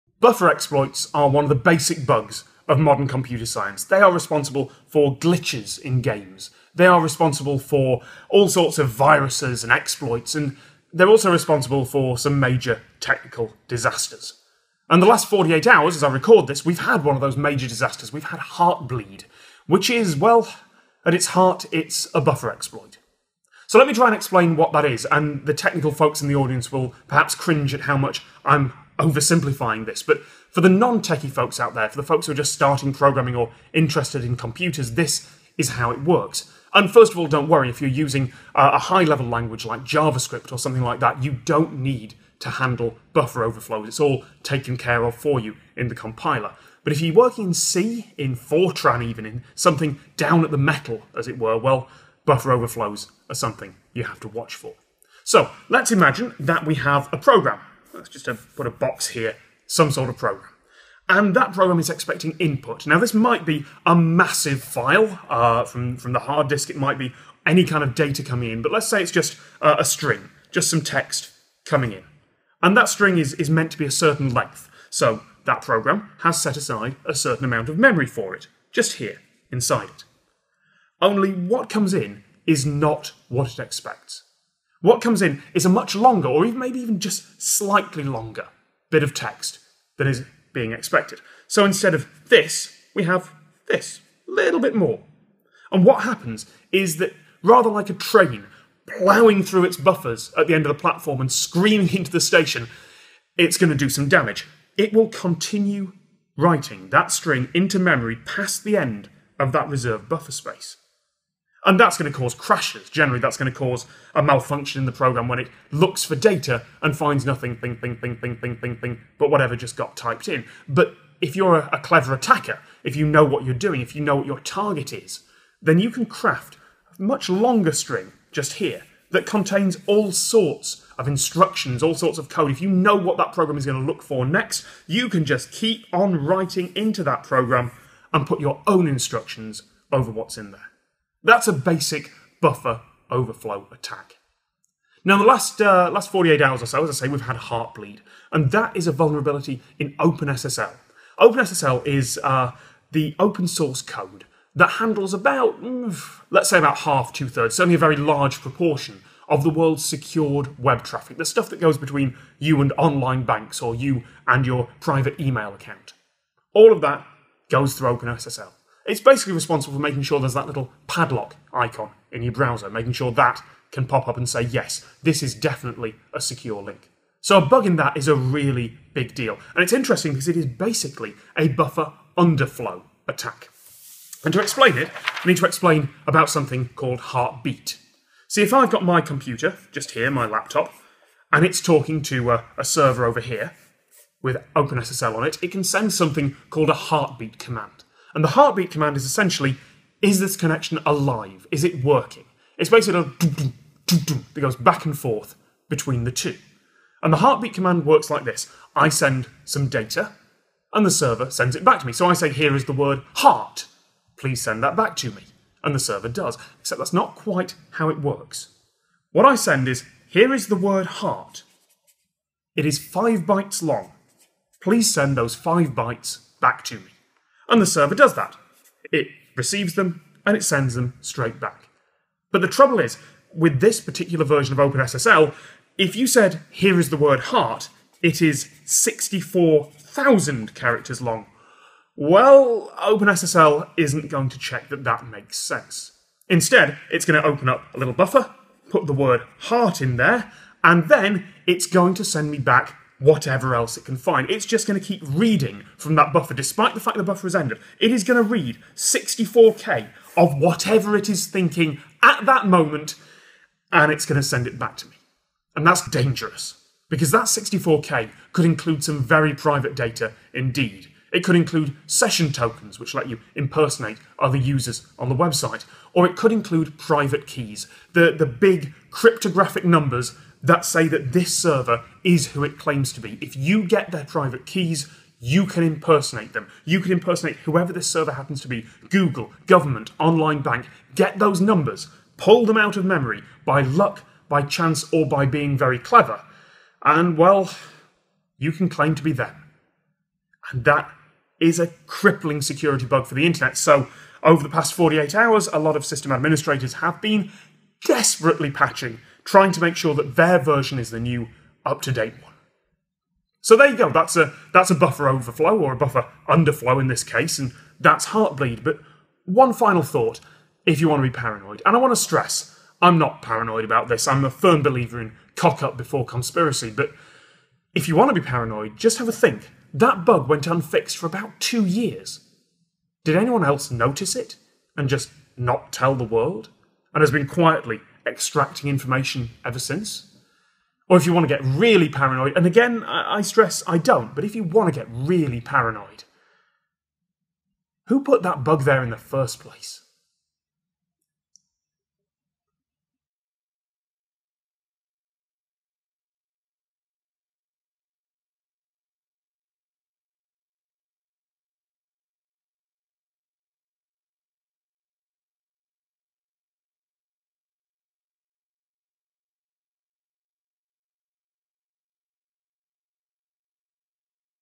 From Missingno to Heartbleed: Buffer Exploits and Buffer Overflows
Buffer exploits are one of the basic bugs of computer science. They're responsible for glitches in games, for all sorts of viruses and exploits, and any number of technical disasters. Here's the basics of how they work, and a non-technical breakdown of Heartbleed, this week's rather startling attack.